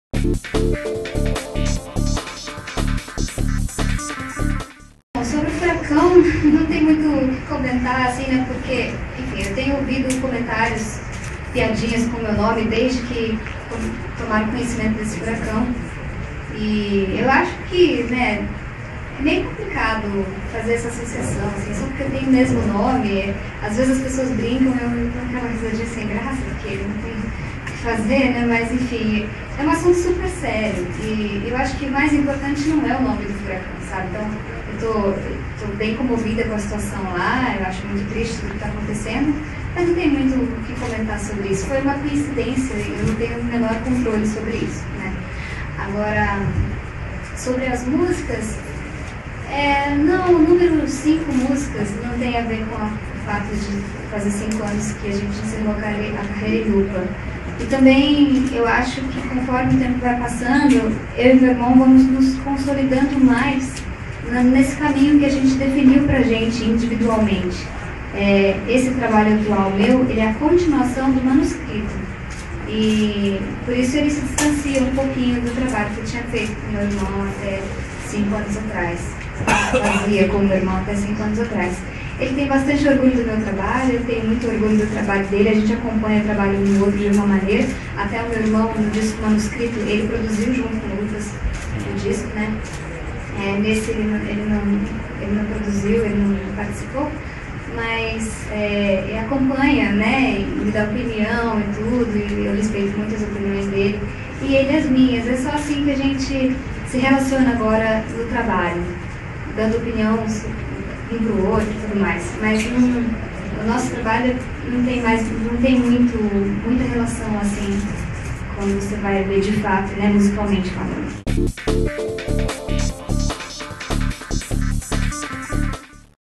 Cantora Sandy vira alvo de brincadeiras por causa do furacão
Artista brasileira diz estar comovida com a situação dos americanos que sofreram com o desastre